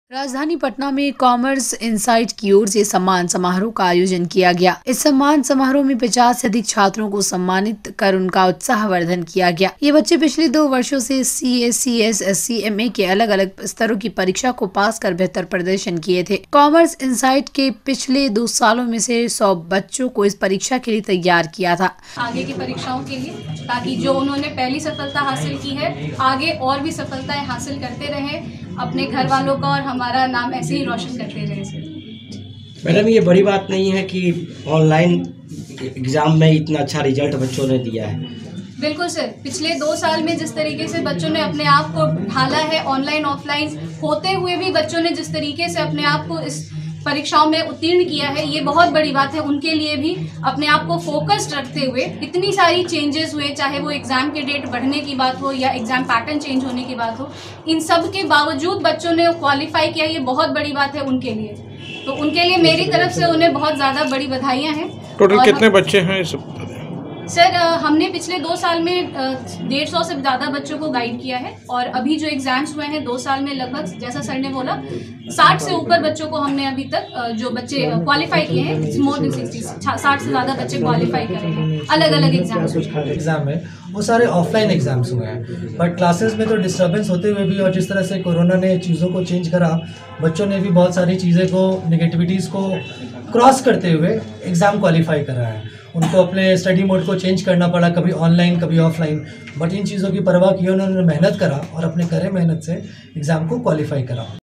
पटना : 50 से अधिक छात्रों को किया गया सम्मानित
पटना : राजधानी पटना में कॉमर्स इनसाइट की ओर से सम्मान समारोह का आयोजन किया गया। इस सम्मान समारोह में 50 से अधिक छात्रो को सम्मानित उनका उत्साहवर्धन किया गया।

यह बच्चे पिछले 2 वर्षो से सीए, सीएस, सीएमए के अलग-अलग स्तरों की परीक्षा को पास कर बेहतर प्रदर्शन किया था। कॉमर्स इनसाइड ने पिछले दो सालों में सौ बच्चों को इस परीक्षा के लिए तैयार किया था।

संस्था की संस्थापक पल्लवी झा ने बताया पिछले साल छात्रों ने बेहतर प्रदर्शन किया था और चार सौ में से 250 से अधिक अंक लाकर जिन छात्रों ने बेहतर प्र’दर्शन किया है उन छात्रों को सम्मानित किया गया है।